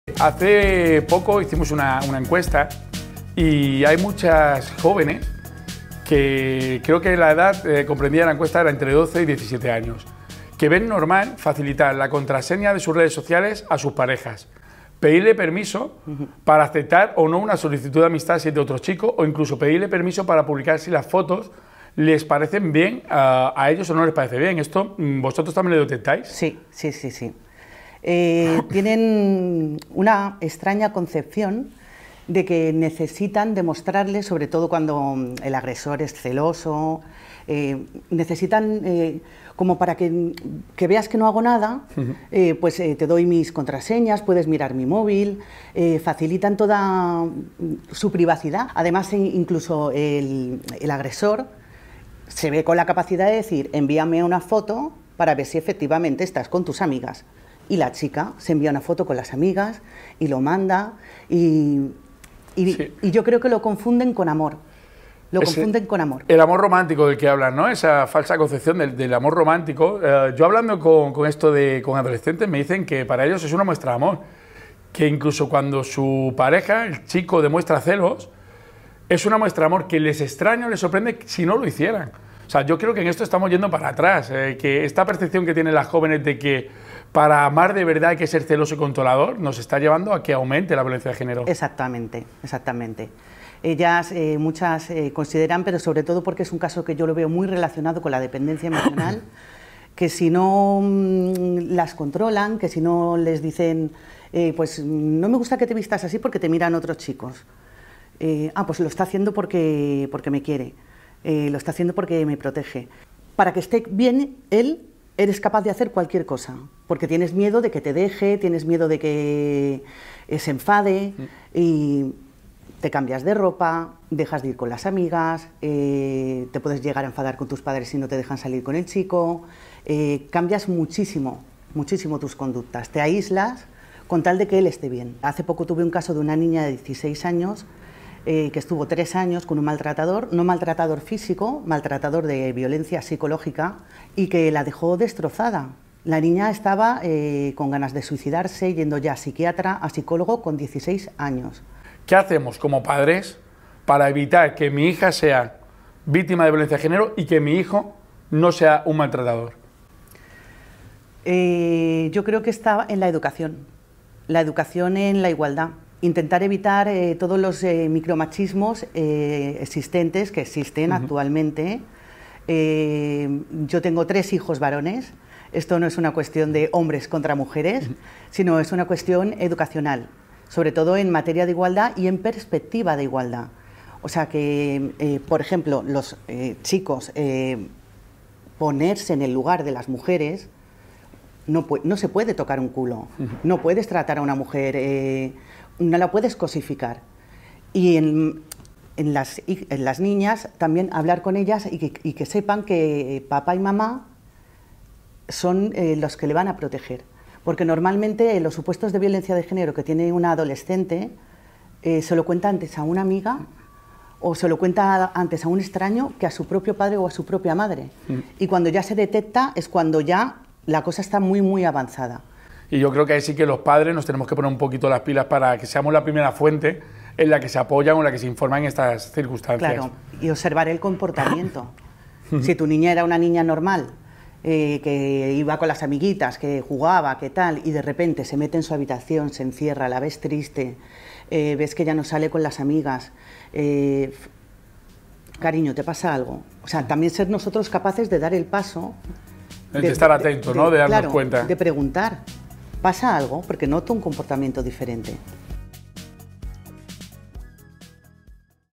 Educando Proteges | Violencia de género, con Raquel Solano
La fiscal decana del juzgado de Manacor, Raquel Solano, habla en Educando Proteges sobre la violencia psicológica de género y la dificultad para detectarla.